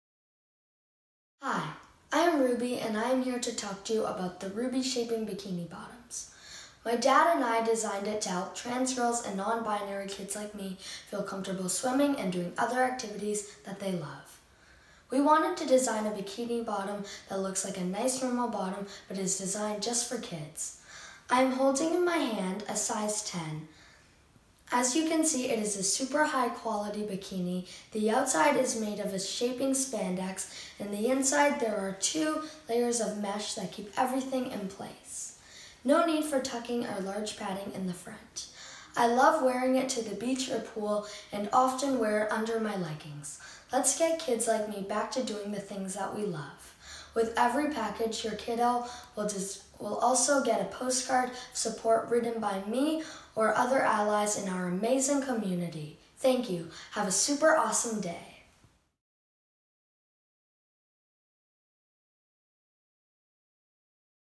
💖🏳️‍🌈 The Ruby Shaping Bikini Bottom product demo by ... Ruby 🏳️‍🌈💖
Ruby's show and tell on how the RUBIES bikini bottoms works to keep non-binary kids and trans girls like her confident and comfortable in the pool or at the beach.